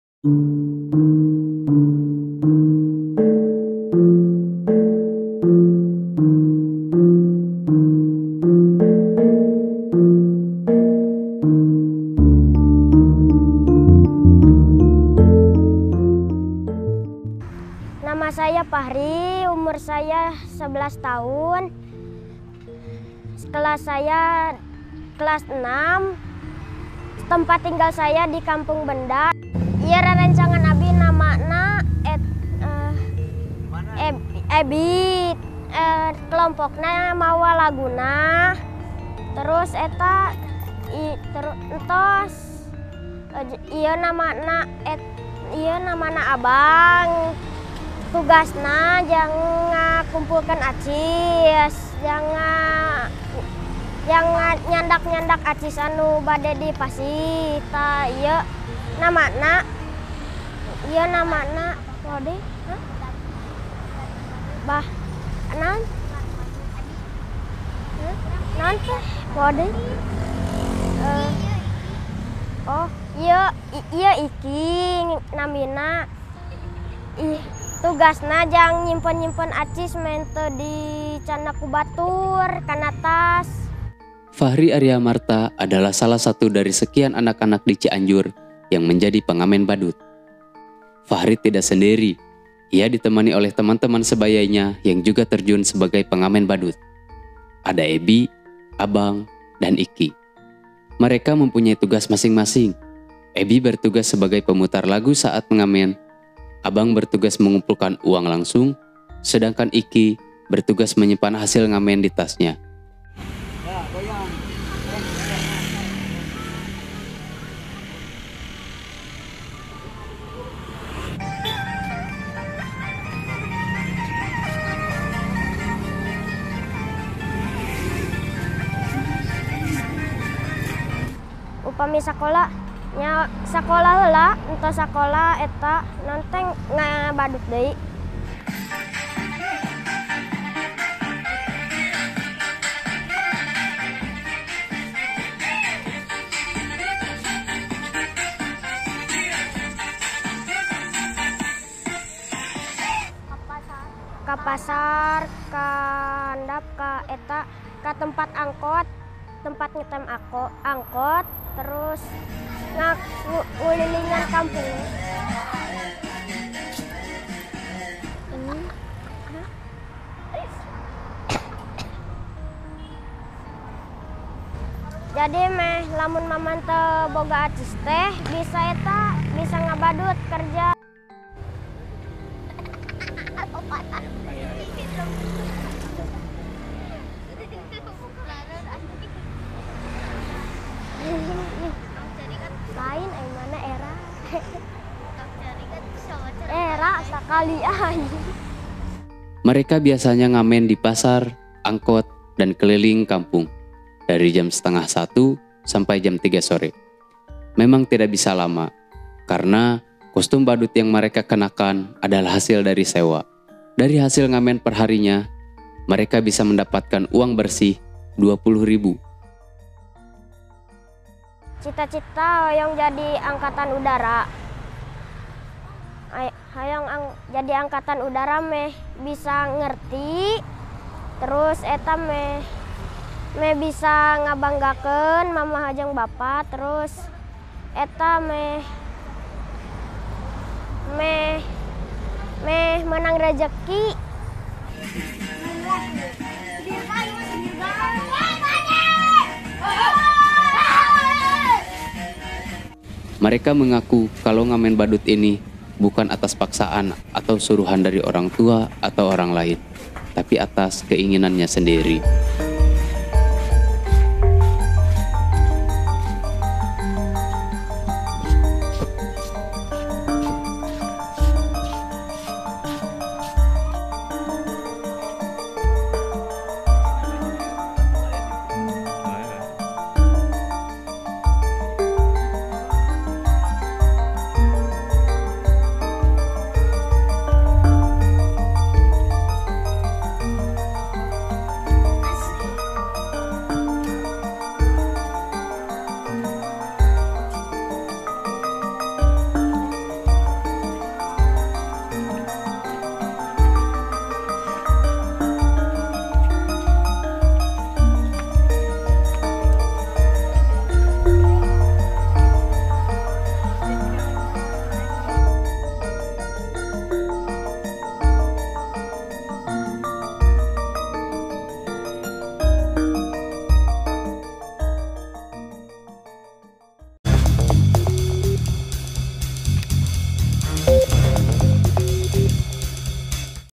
Keliling Cianjur untuk 5000 Rupiah
Ini adalah segmen Joernal dari Channel Biasa.
Di segmen ini, kami mencoba mengangkat dokumenter tentang Fahri dan teman-temannya yang mengamen Badut dengan keliling kota Cianjur.

penasaran cerita dibalik Fahri ini? tonton video lengkapnya,
jangan ketinggalan Info dan Video selanjutnya, dengan nyalakan lonceng bel notifikasi.